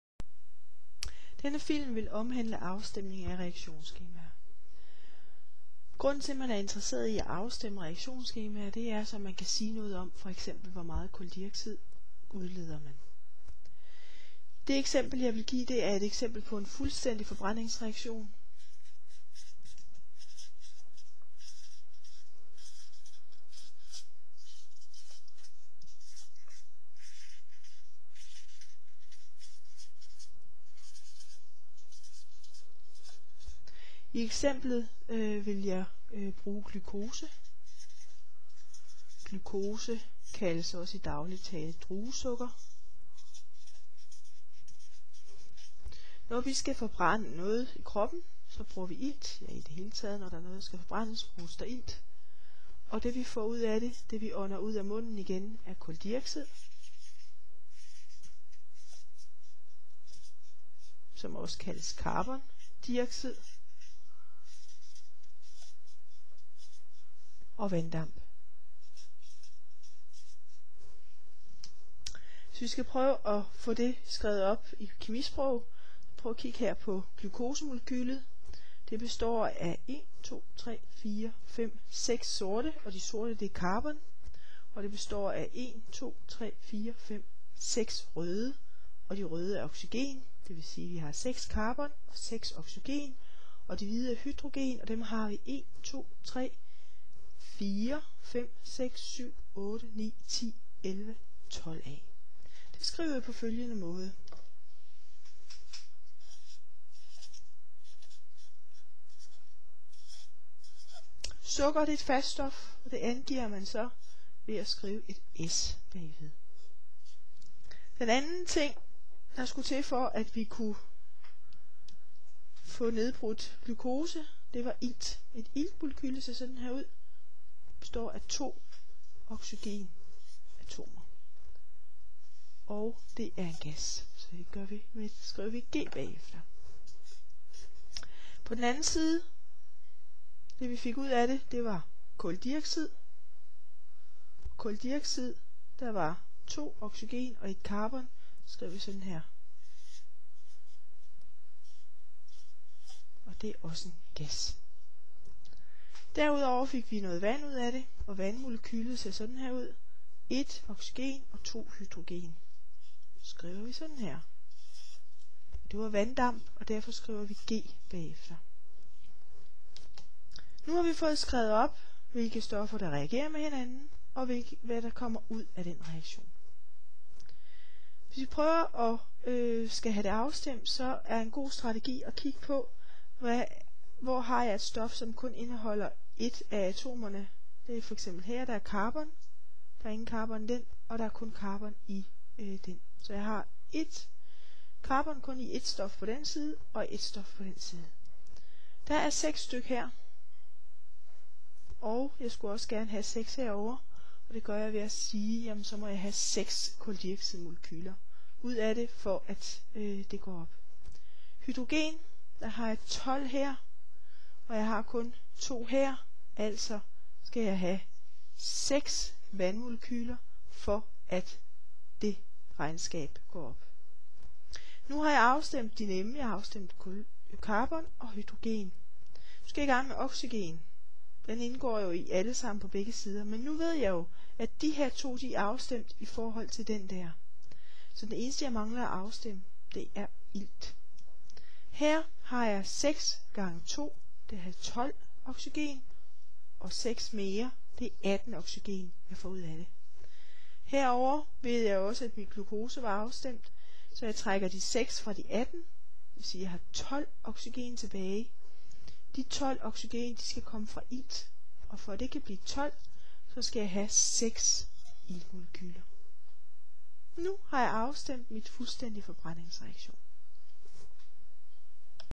Mettes kemi - afstemning af reaktionsskema
Forklaring af hvordan man afstemmer et reaktionsskema